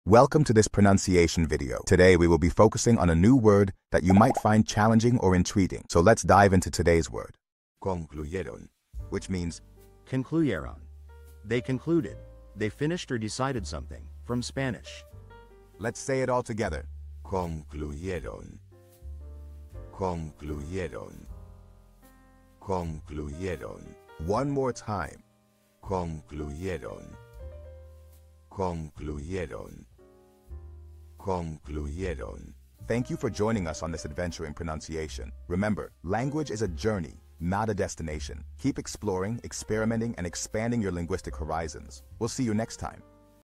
How to pronounce Concluyeron
Master the Pronunciation of 'Concluyeron Which Means concluded' - which means : They concluded: they conclude;They Finished or Decited Subthing (from Spanish). 📝 with @PronunciationSpanish 🎓🗣️ - [Your Guide to Spanish]

Learn to pronounce 'Concluyeron' perfectly in Spanish with our easy and engaging tutorial by @PronunciationSpanish! 📖📽️ In this comprehensive video, we provide you with native speaker examples 🗣️👂 and phonetic breakdowns 🎵 to ensure you get the pronunciation just right.

How to say They concluded in Spanish